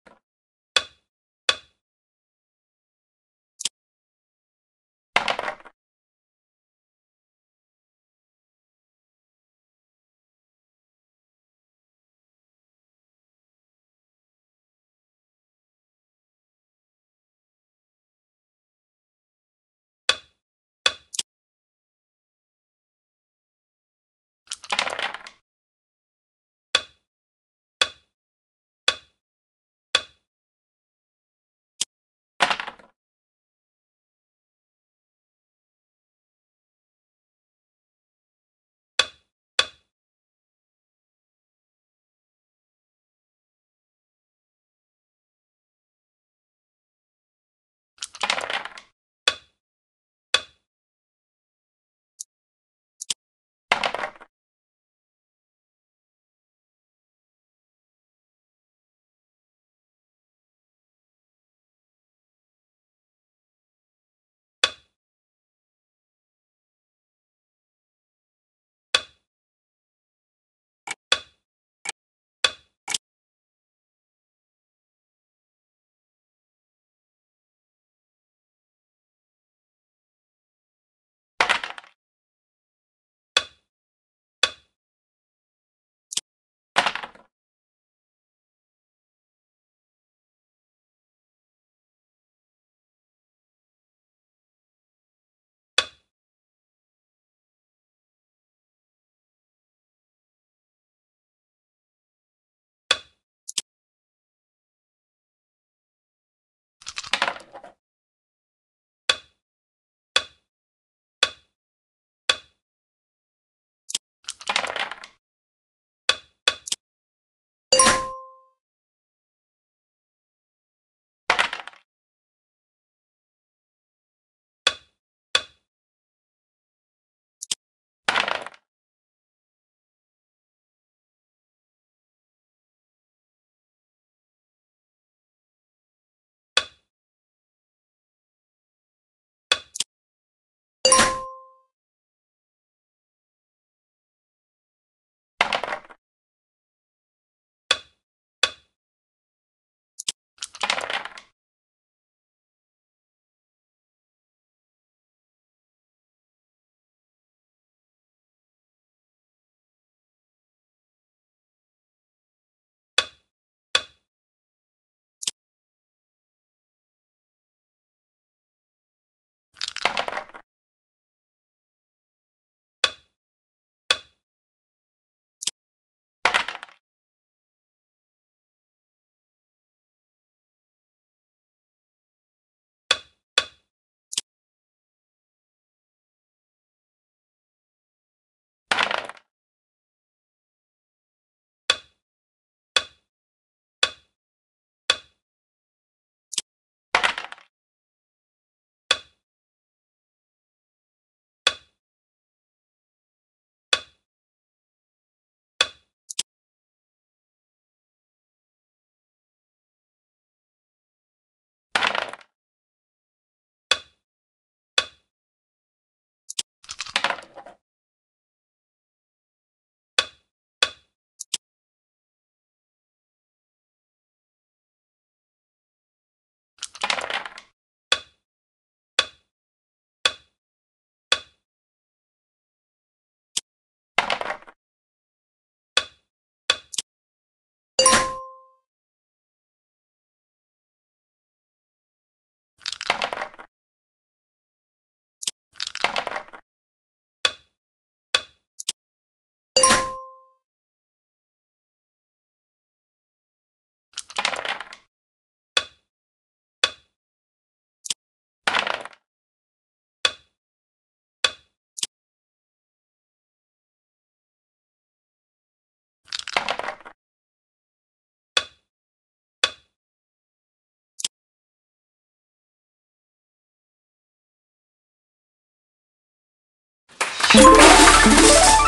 Vezi asta : Un joc de table foarte disputat
Multe piese afara , disputa la maxim , un joc frumos de table